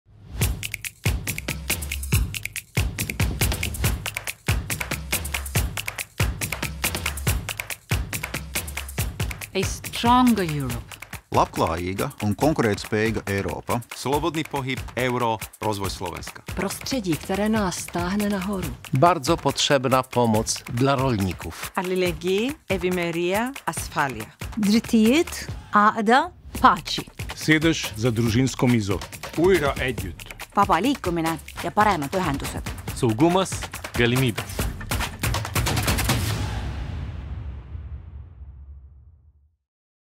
What has the 2004 great enlargement brought to Europeans
20 years ago, our European family grew bigger, with 🇨🇾 Cyprus, 🇨🇿 Czech Republic, 🇪🇪 Estonia, 🇭🇺 Hungary, 🇱🇻 Latvia, 🇱🇹 Lithuania, 🇲🇹 Malta, 🇵🇱 Poland, 🇸🇰 Slovakia, 🇸🇮 Slovenia joining our Union.

Today, 79% of citizens living in these countries believe that their country has benefitted from the European Union membership. And their inclusion has made our Union stronger.

The pace of change across the European Union over the past two decades has been remarkable. Discover how